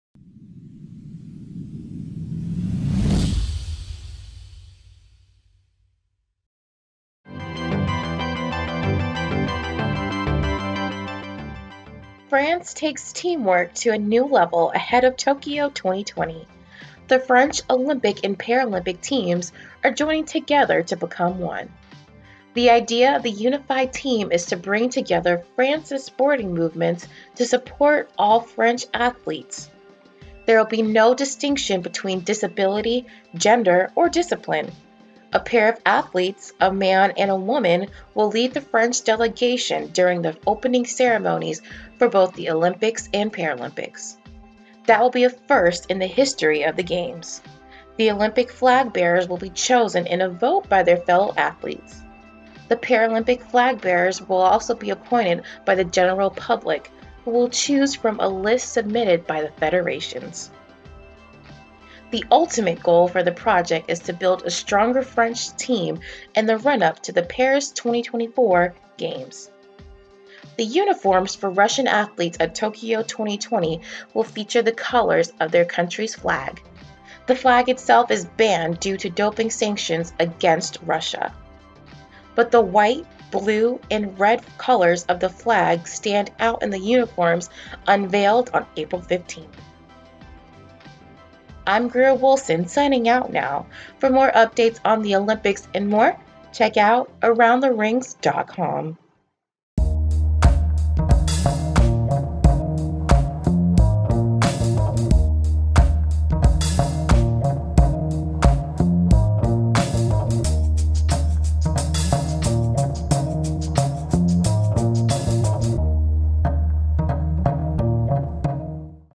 2Minutes - Team France United As One, Russia Unveils Uniforms
For Tokyo 2020, Team France says all for one. The French Olympic and Paralympic teams are merging as one, in order to support French athletes. For the first time in Olympic Games history---A man and a woman, will lead the French delegation during the opening ceremonies for both the Olympics and Paralympics. Also, Team Russia unveils uniforms for Tokyo 2020.